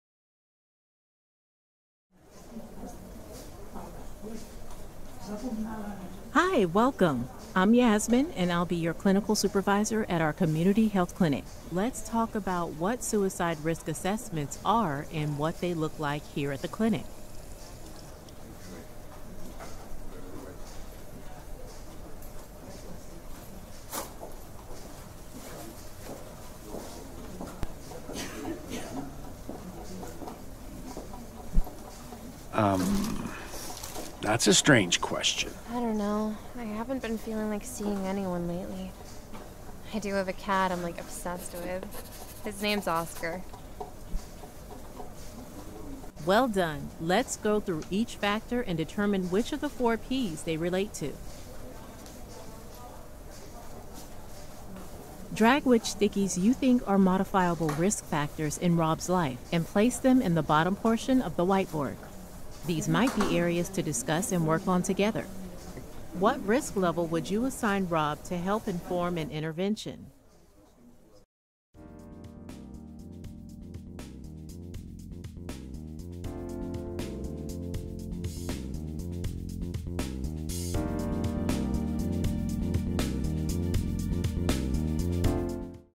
CAMH Virtual Reality (VR) suicide risk assessment training
The CAMH Simulation Centre has designed and developed an innovative Virtual Reality (VR) suicide risk assessment training. This training is targeted towards medical, nursing, and interprofessional students and new-to-practice clinicians as a training on how to conduct a suicide risk assessment that is based on national guidelines and best practices.

This VR suicide risk assessment training offers participants the opportunity to complete a suicide risk assessment with two different avatar patients. Both patient scenarios include an avatar preceptor, who guides participants through the VR scenario from beginning to end; the preceptor initially teaches participants the theoretical suicide risk assessment framework used in the game, and works with learners to reflect and categorize the risk and protective factors they uncovered during their patient interactions, ending with identifying the level of risk for each avatar patient.

This VR training is also offered across two modalities; an in-person group training where participants don virtual reality headsets and engage in a facilitator led debrief, and an asynchronous individual option where learners can complete the training remotely on their computers on their own time.

This project was funded through eCampusOntario. If you work in a post-secondary institution, you can access the full training through the eCampusOntario open library